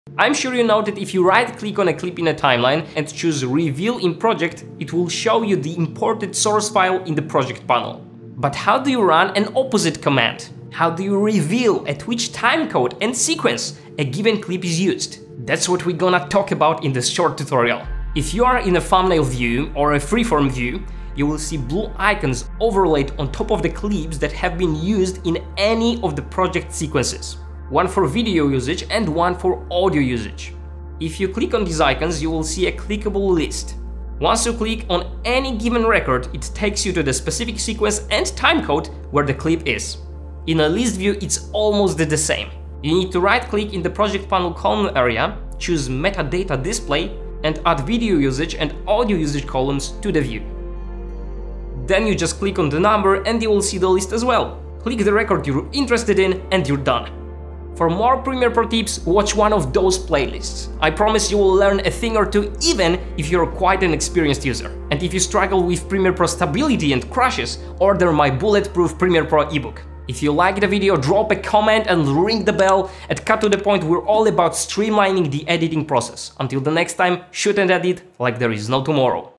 How to Find a Clip in a Sequence [Hidden Gem 😎]
Find out how to reveal at what timecode and sequence a given clip was used.

MUSIC provider I use (with this link you get 2 additional months to your subscription)

STOCK FOOTAGE service I use (with this link you get 2 additional months to your subscription)